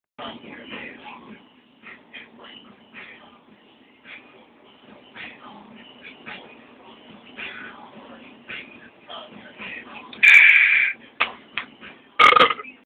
Beer chug & burp!
Chug a beer, smash can on head, burp.